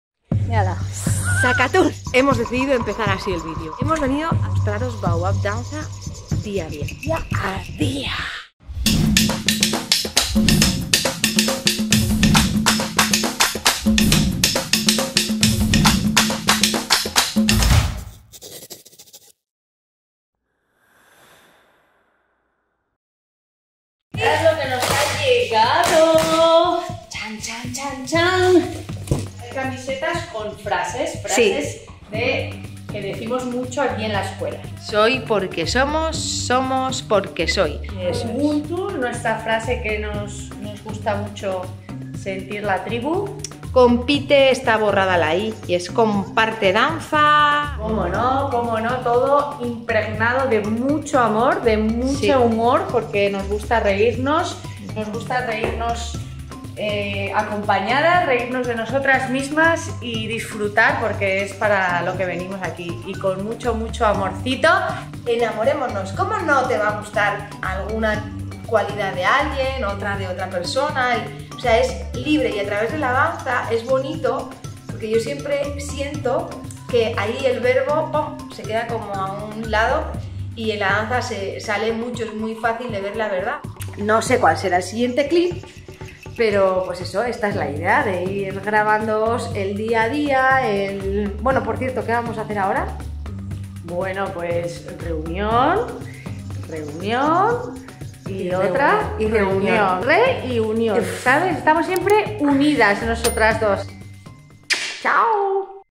Día a día 1 | Camisetas en el Baobab
Querida Tribu ya tenemos las camisetas a vuestra disposición. Si quieres regalar mensajes no dudes en ponerte en contacto con nosotras.

Tenemos muchas ganas de ir mostrando aquí nuestro día a día, nuestro cotidiano.

Coméntanos lo que quieras, estamos deseando leerte, leeros.

Empezamos aquí este tramo de camino, veamos a donde nos lleva🥰